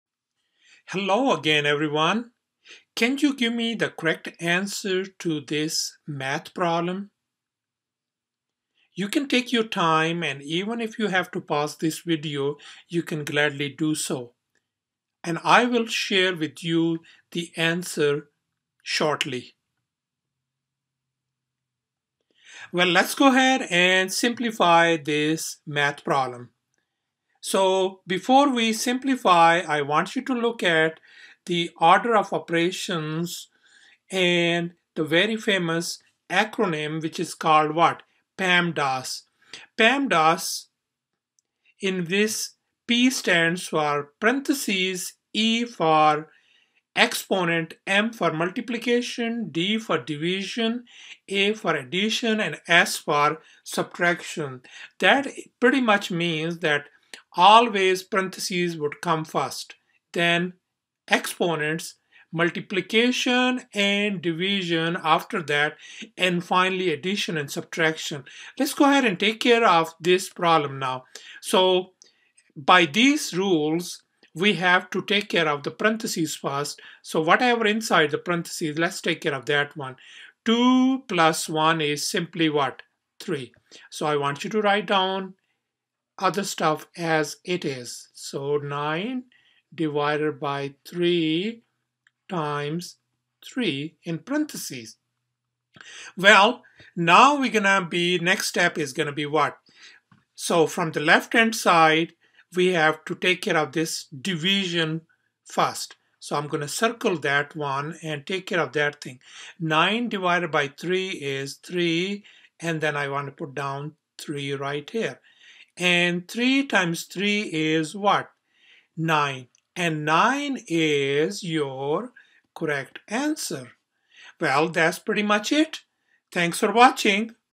Social Media Math Problem: Order of Operations #2
Can you solve this Social Media Math Problem using the Order of Operations? PreMath.com